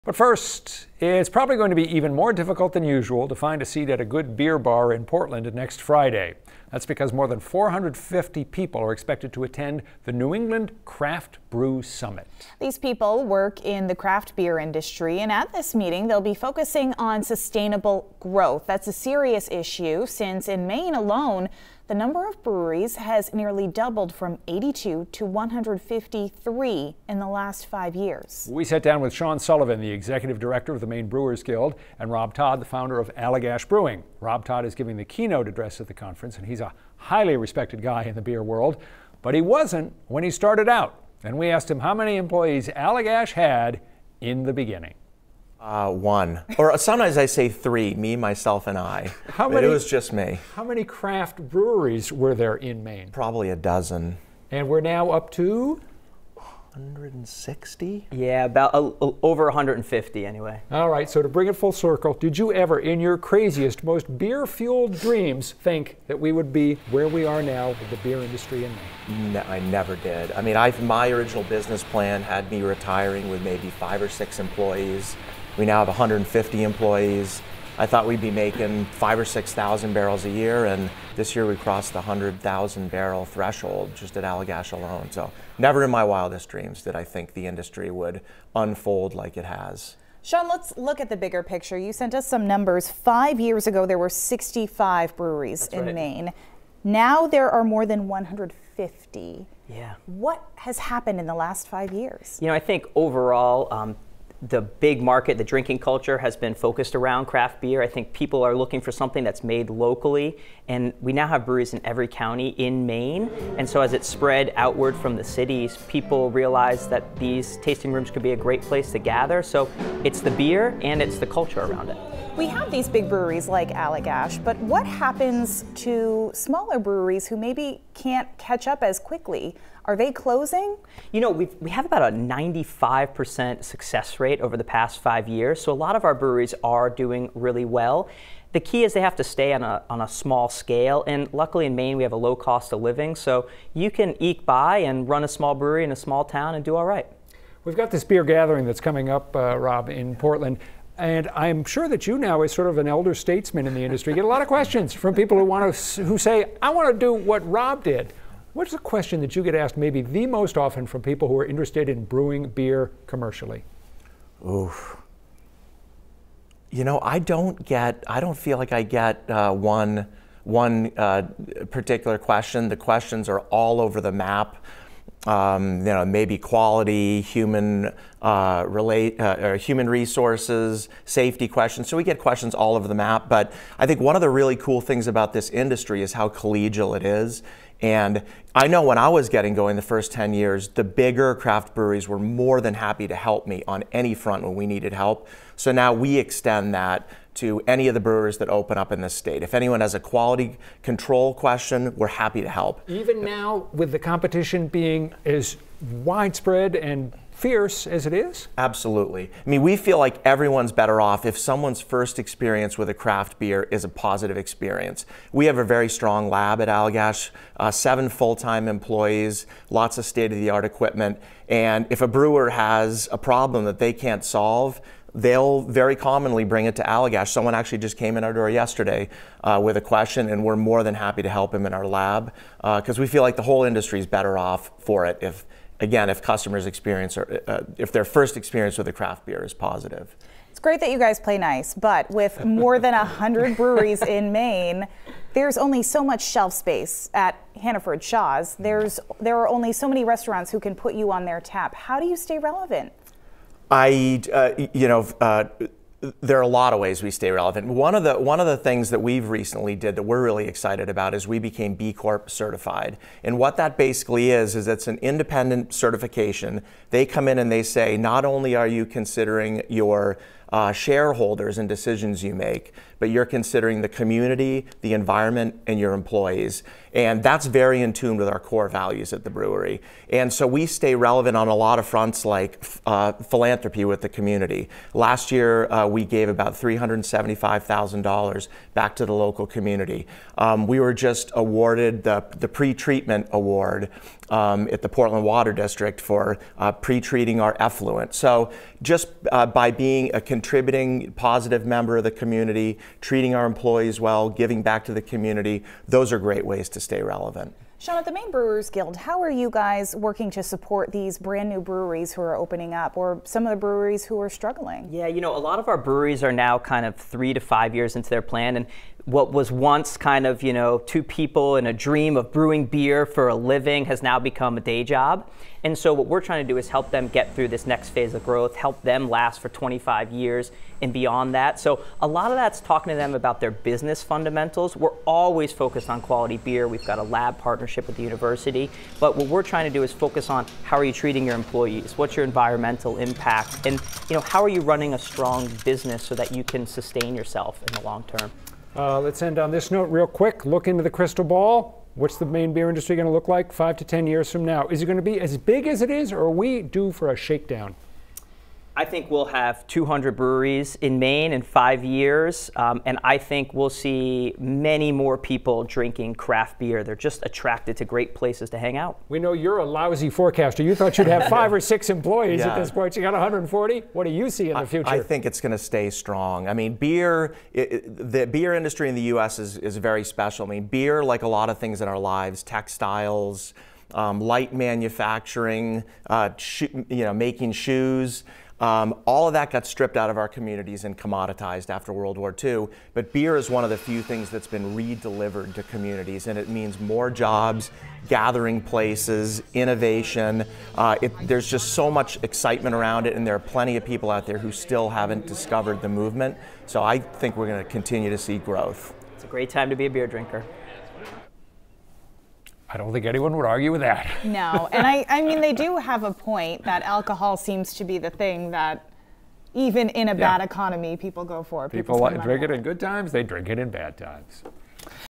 The Maine Craft Beer Boom, How Far Can It Go?
In four decades, we’ve gone from one brewery to more than 150